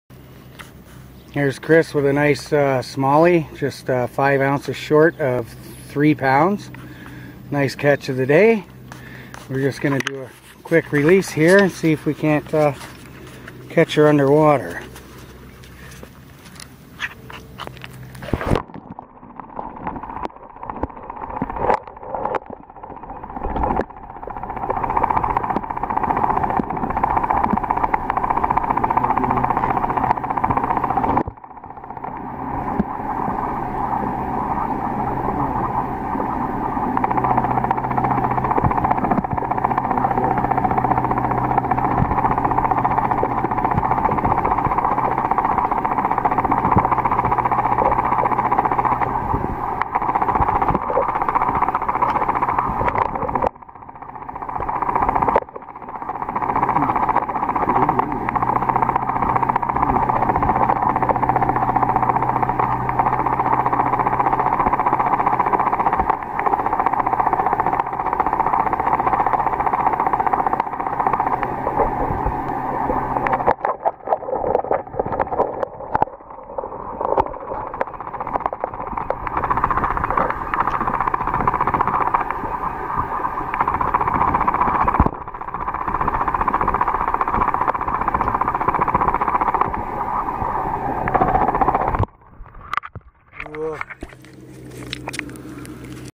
Small Mouth Bass Fishing Pier 4 Hamilton, ON
Chris Bertin's 3lb smallmouth bass